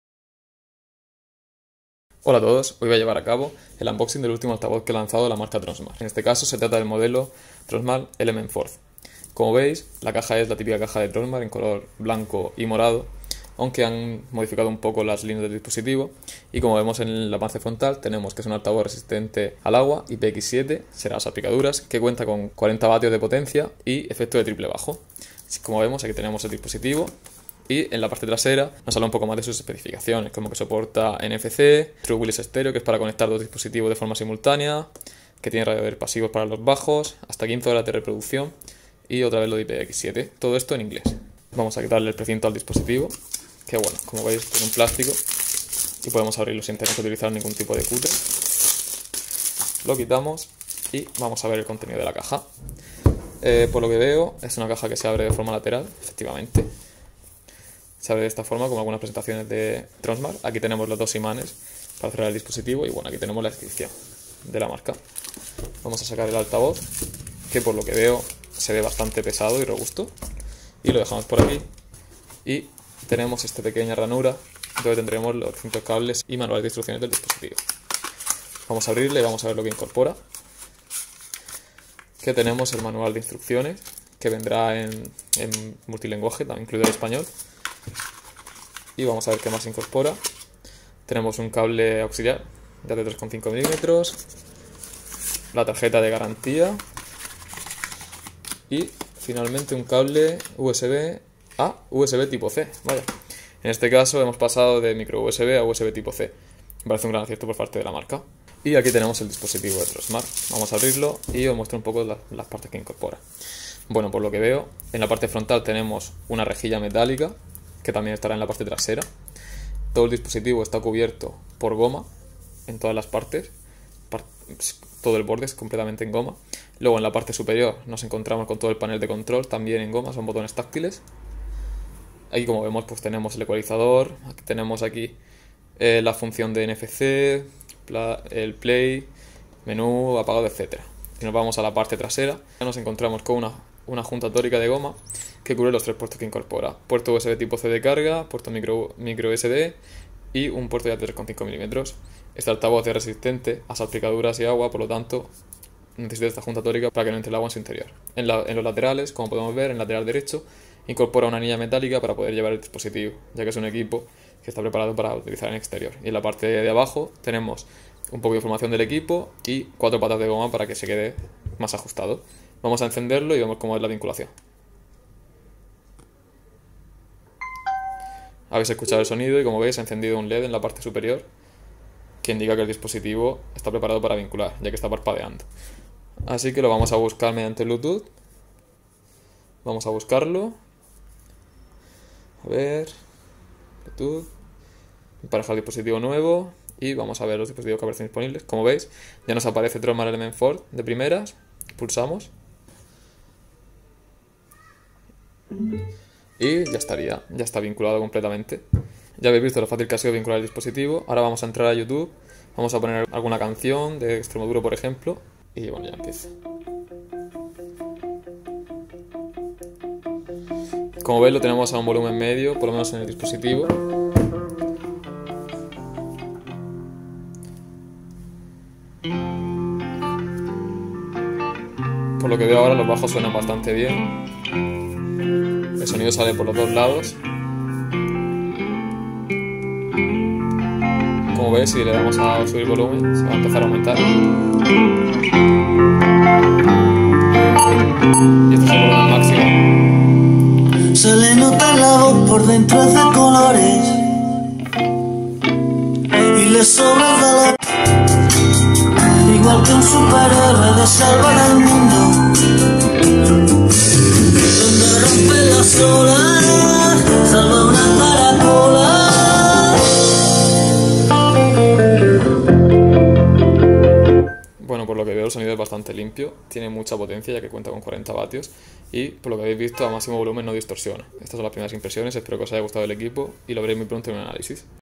Tronsmart FORCE - Unboxing y primeras impresiones
¡Unboxing y primeras impresiones del nuevo altavoz de Tronsmart!
La renovación de Tronsmart MEGA vuelve con un equipo más versatil, que ahora puede sumergirse y cuenta con novedades como USB Tipo-C y equializador

¡No os perdáis las primeras impresiones!

¡SIGUENOS!